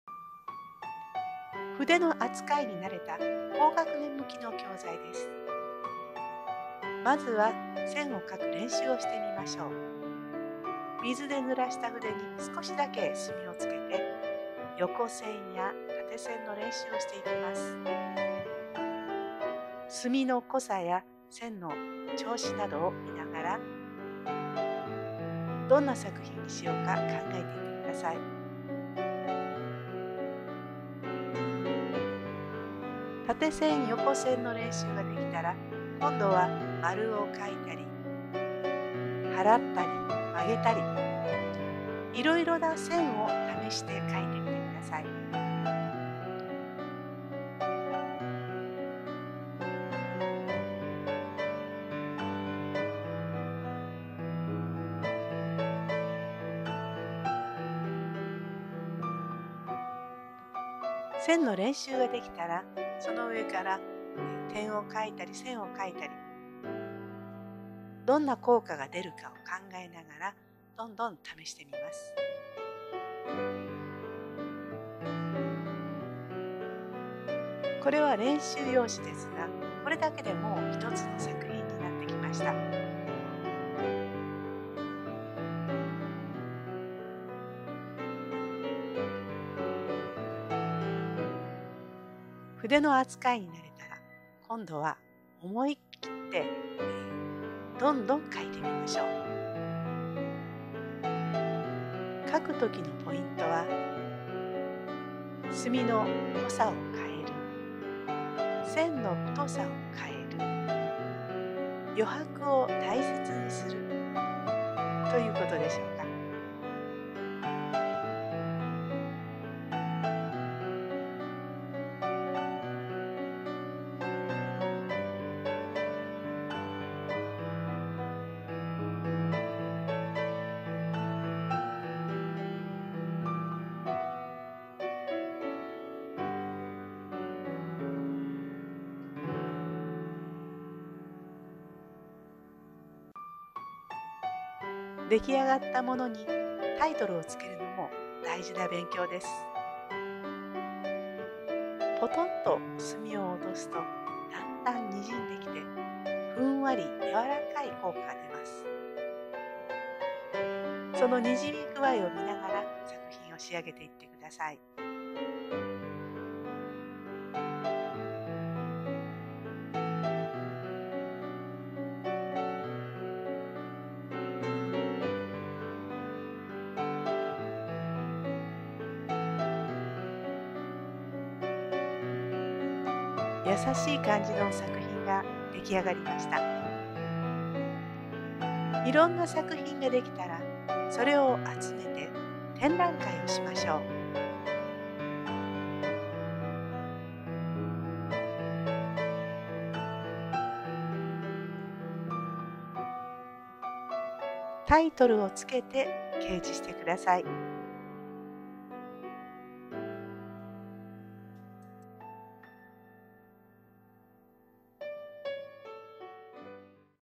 kimie gangiの図工教室 墨で描く(簡単･１時間でできる)
毛筆の扱いに慣れた高学年向きの教材です。１～２時間でできるので、４月の授業参観の背面掲示にぴったりです。
具体物を描くのではなく、思いのままに描くことを楽しんでください。
できあがった作品にはタイトルを付けます。
初めはどうしたらいいか分からない子も、友達と一緒にイメージに合うタイトルを考えることで、作品の見方が自然と身についていきます。
できあがった作品を展示することで、様々な線のよさに気づき、今後の制作活動にも役立つことでしょう。
雁木君江(がんぎきみえ)